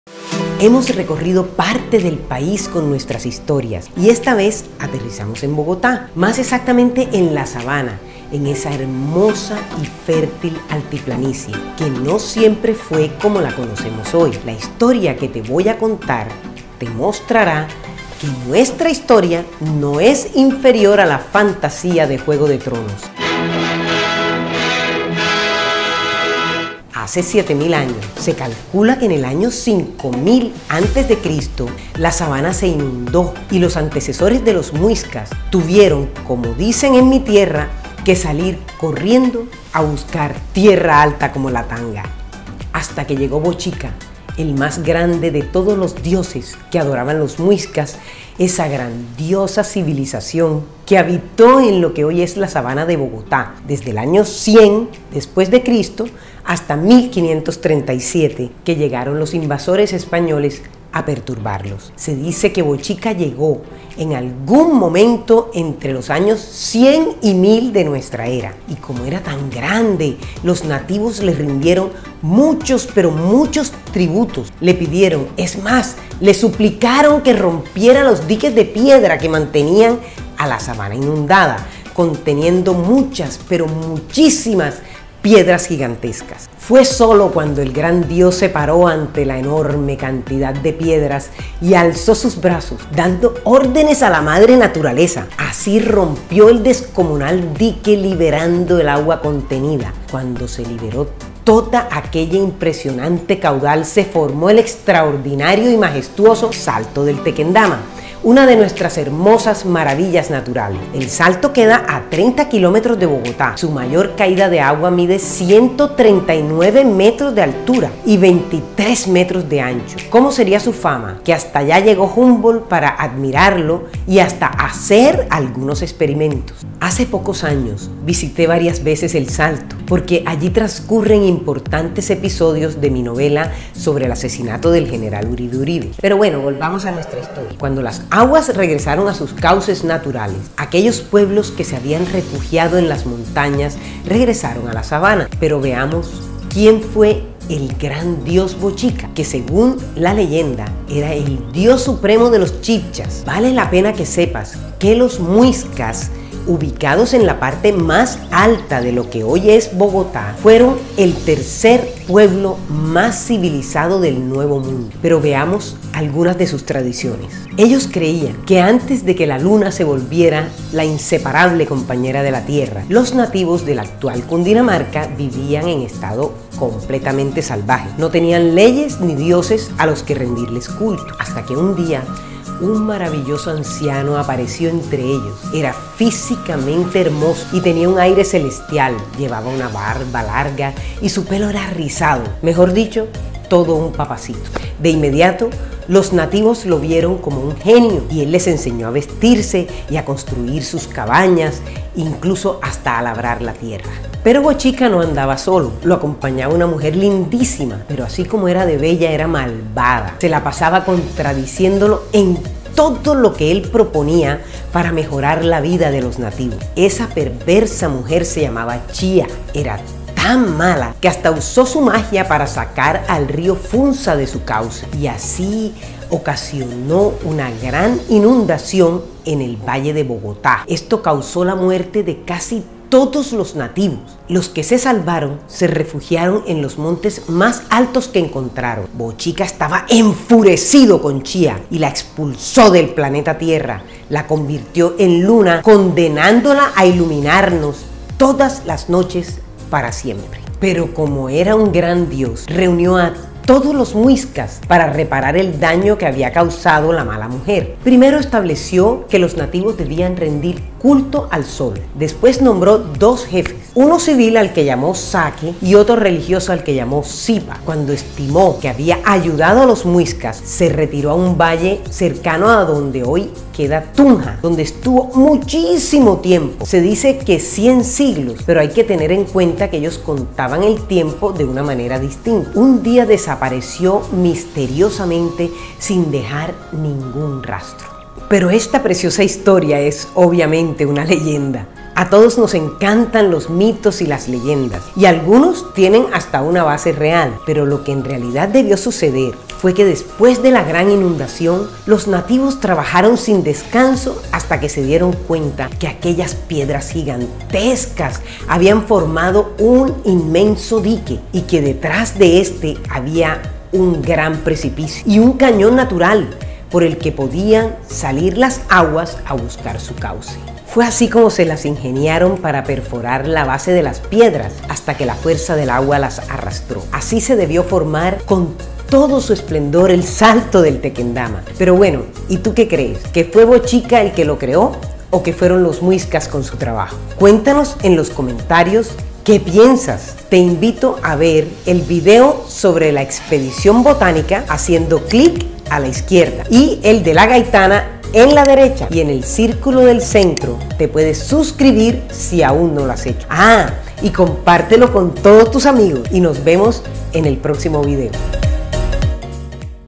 Bochica, el más grande de los dioses mwiskas
Bochica fue el más grande de los dioses que adoraban los Mwiskas, se dice que llegó, en algún momento, entre los años 100 y 1000, de nuestra era; y como era tan grande, los nativos le rindieron muchos tributos, fueron ellos quienes le pidieron que rompiera los diques de piedra que mantenían la Sabana inundada, desde el año 5.000 a.C.

Se dice que, Bochica se paró ante las gigantescas piedras, alzó sus brazos, y dando orden a la madre naturaleza, rompió el dique, liberando toda el agua contenida.

Así se creó el Salto del Tequendama…

Fuentes:
Santos Molano, Enrique. Las grandes noticias colombianas.
Conde de las Casas. Atlas Histórico, Genealógico, Cronologico, Geográfico y Estadistico de Lesage. Editado en 1826.

Dirección general y presentación: Adelina Covo
Guion: Enrique Santos Molano y Adelina Covo
Producción y edición: @kactusfilms